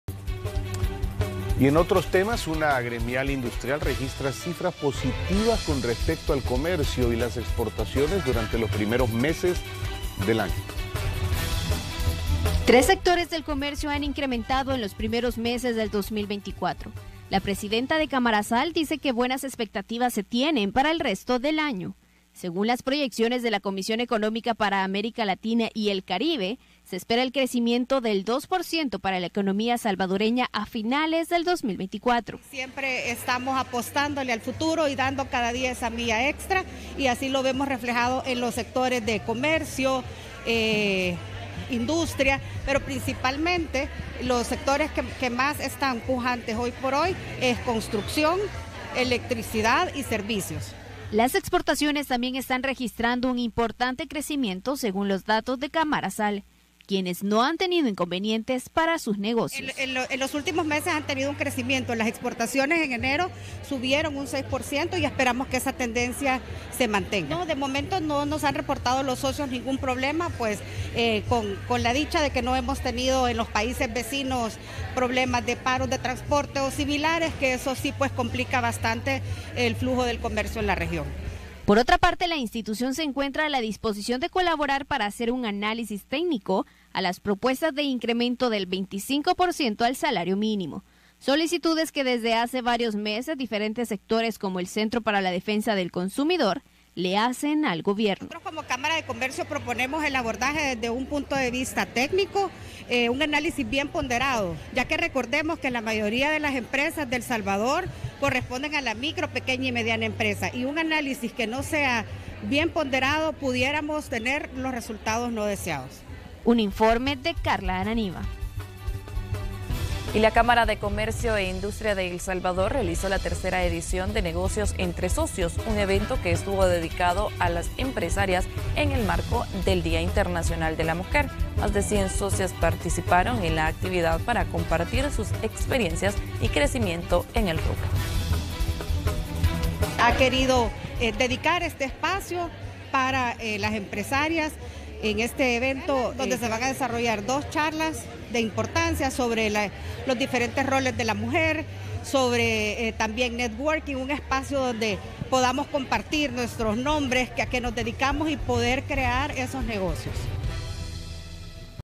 Incrementa del comercio y las exportaciones para este 2024
La presidenta de Camarasal dice que buenas expectativas se tienen para el resto del año, según las proyecciones de la Comisión Económica para América Latina y el Caribe.